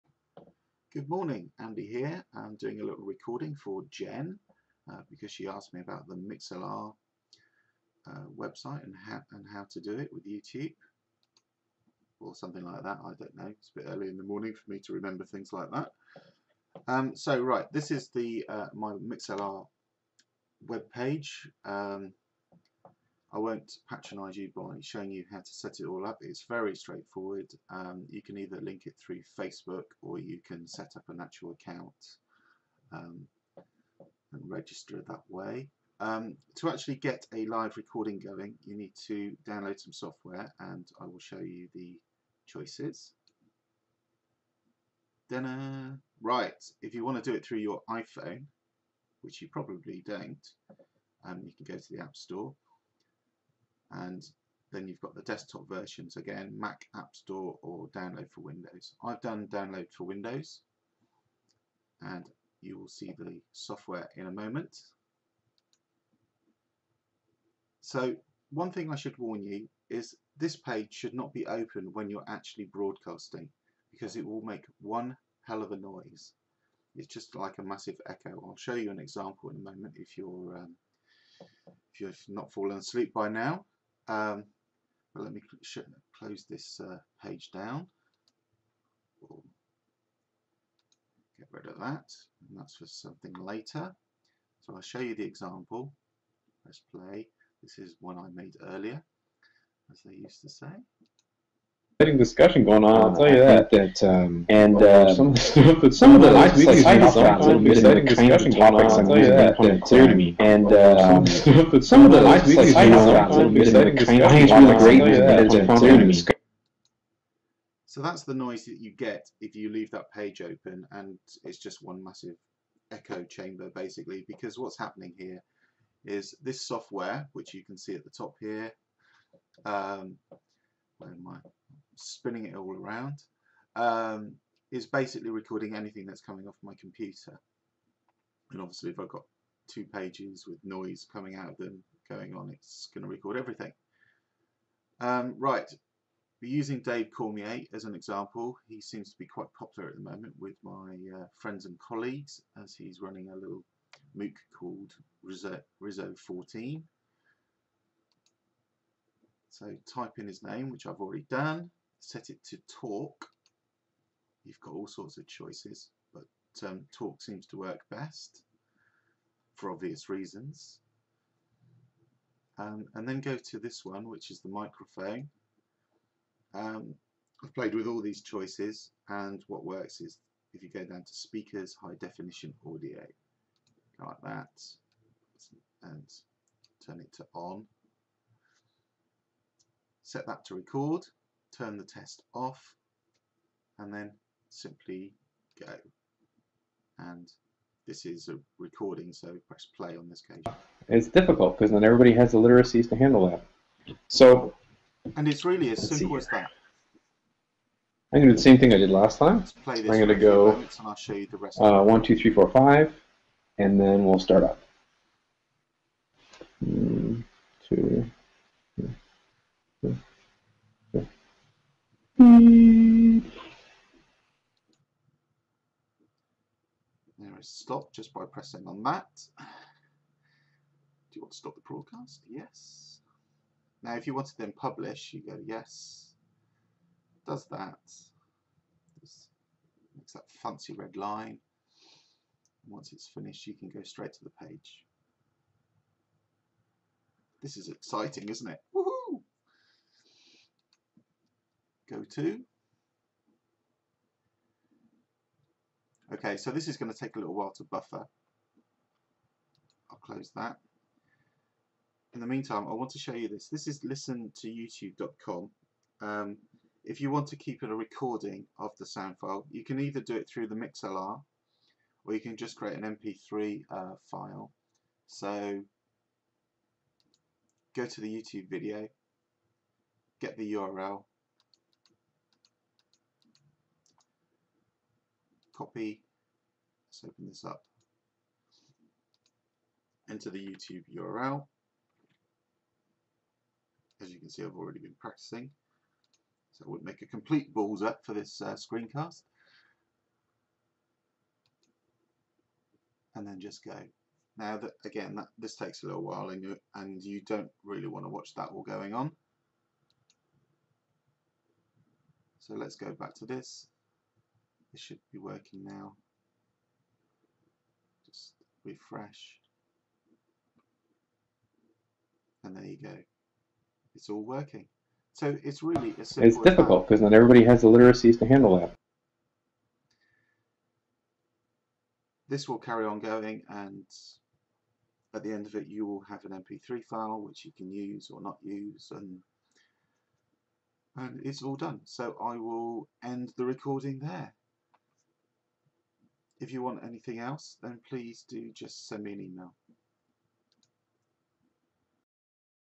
Streaming live audio via Mixlr for those who can't access Youtube
Using a Dave Cormier recording as an example, this quick screencast shows how to set up Mixlr for streaming live audio.